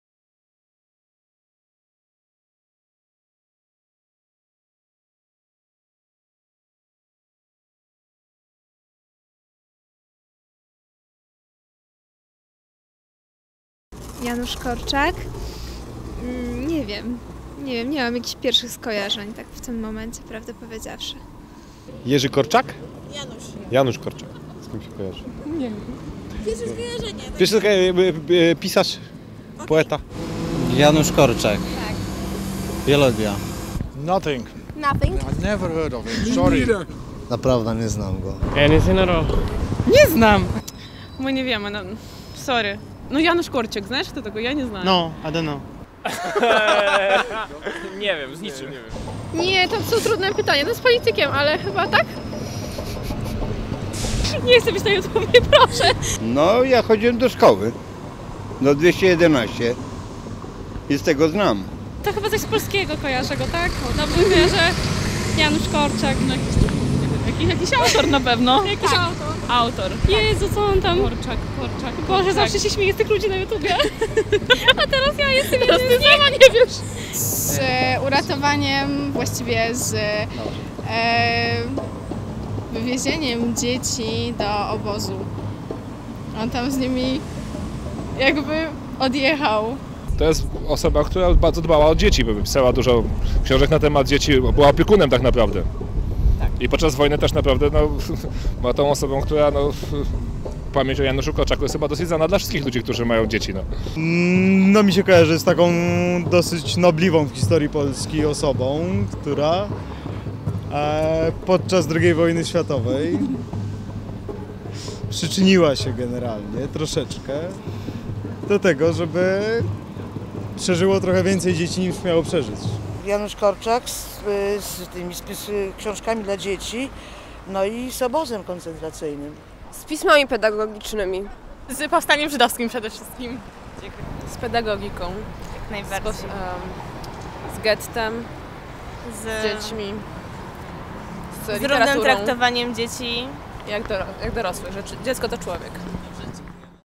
Z czym kojarzy Ci się Janusz Korczak? Sonda uliczna.
Sonda została zrealizowana na ulicach Warszawy przez uczniów Gimnazjum TAK w Opolu w ramach "Projektu filmowego: Korczak". Projekt miał na celu zaznajomienie naszych gimnazjalistów z sylwetką Janusza Korczaka.

Podoba Ci się film? Polub nas na FB